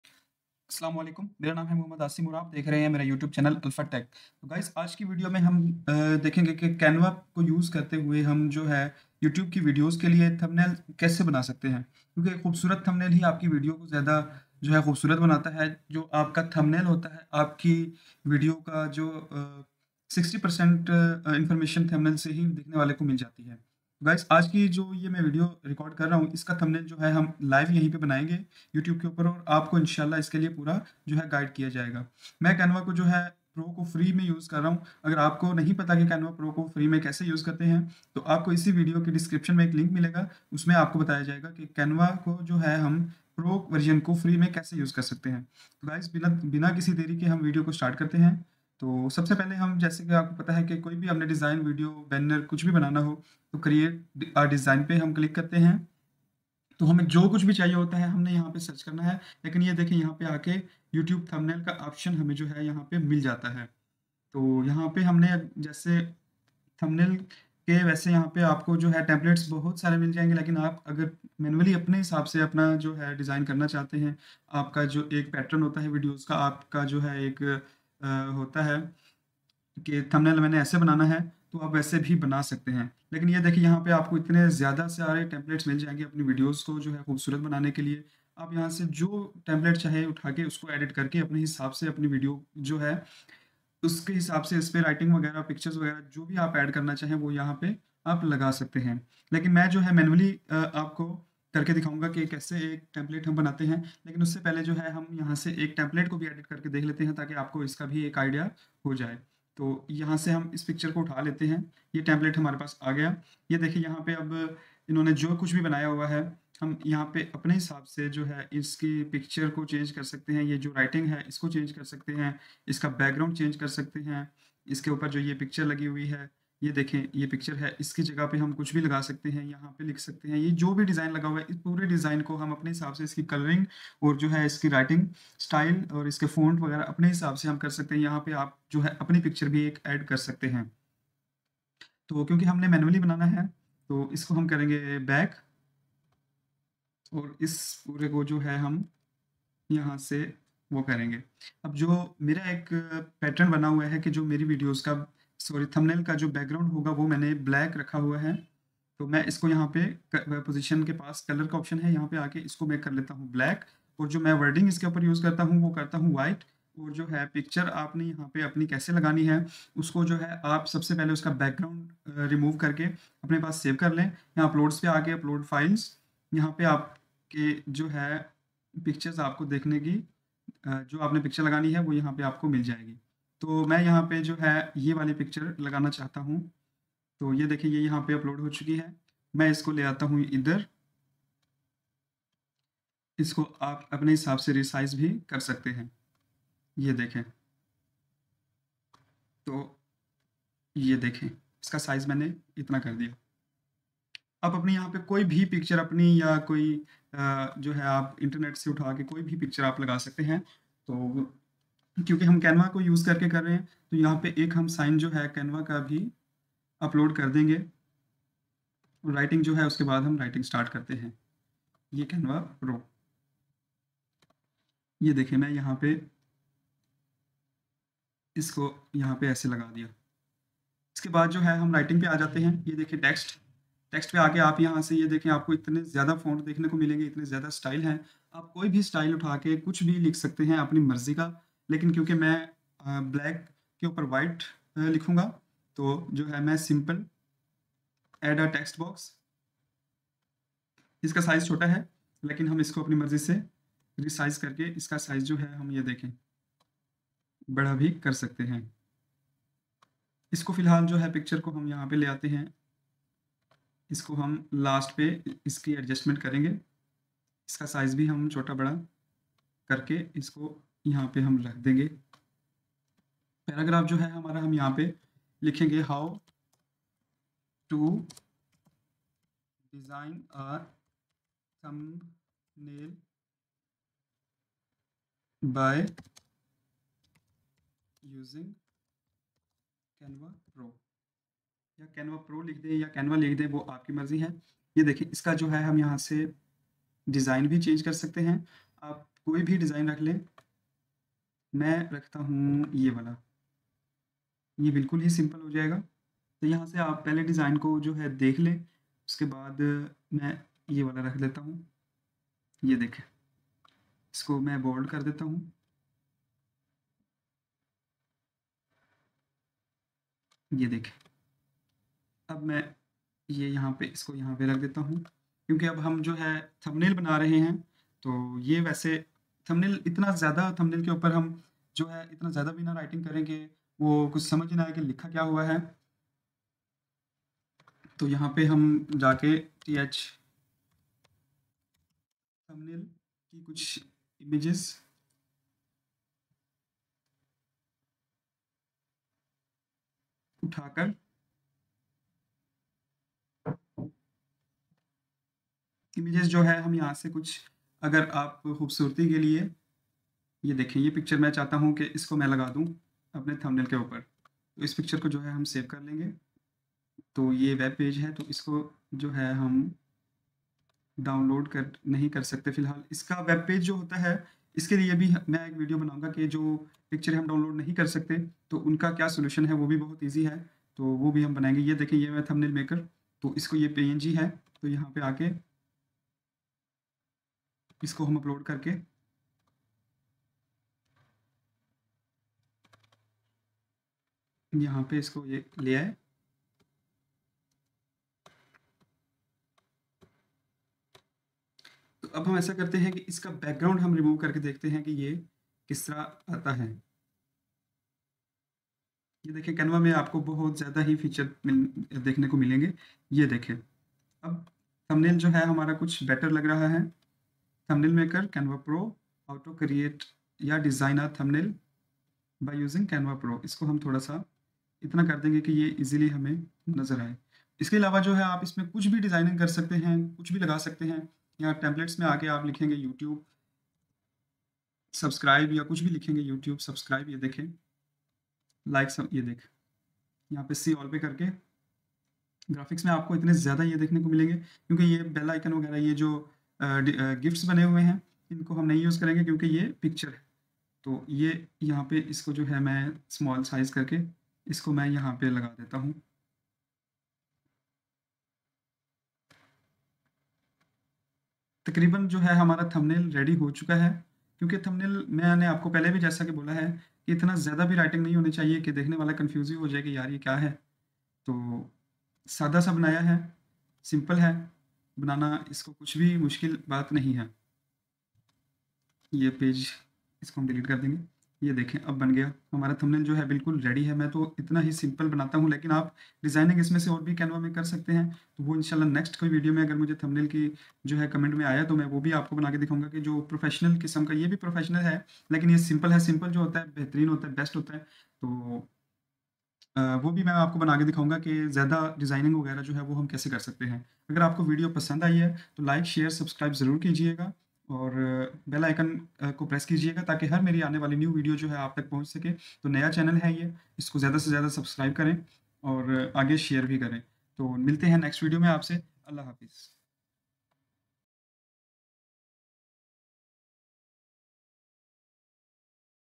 Thumbnail Kaise Banaen | Free YouTube Thumbnail Maker | YouTube Thumbnail Template
Assalam-o-Alaikum
Are you tired of your YouTube videos going unnoticed? The secret lies in creating eye-catching thumbnails that grab viewers' attention and entice them to click. In this video, we'll guide you through the process of designing captivating thumbnails that will boost your channel's visibility. From composition and color selection to typography and graphics, we'll reveal expert tips and tricks to make your thumbnails stand out from the crowd. Whether you're a beginner or seasoned creator, this tutorial will equip you with the essential knowledge to design compelling thumbnails that drive more views, clicks, and ultimately, the success of your YouTube channel. Don't miss out on this opportunity to take your content to the next level.

How to Get Canva Pro For Free ;